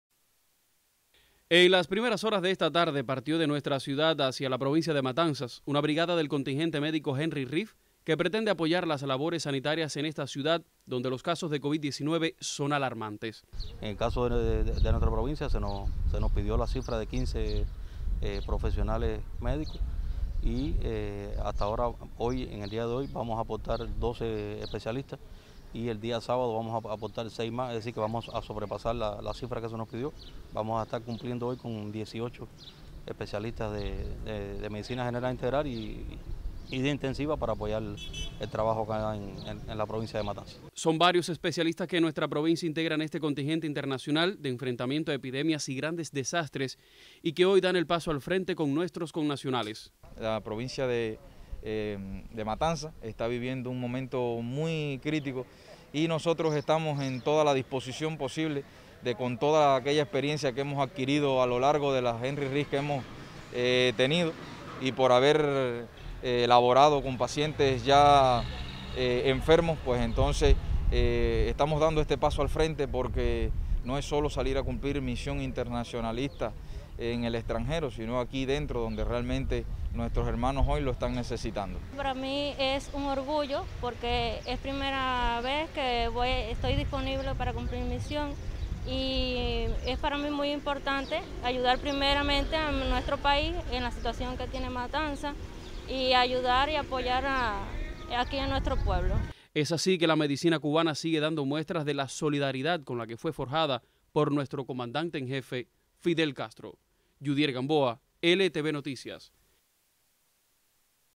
Brigada de médicos tuneros apoya enfrentamiento a la pandemia en Matanzas
Una primera parte de médicos tuneros viajan  a la provincia de Matanzas para enfrentar la situación sanitaria  impuesta por la Covid-19. Ante el llamado de la máxima dirección del país, un total de 18 galenos nuestros estarán en la primera línea de enfrentamiento a la pandemia.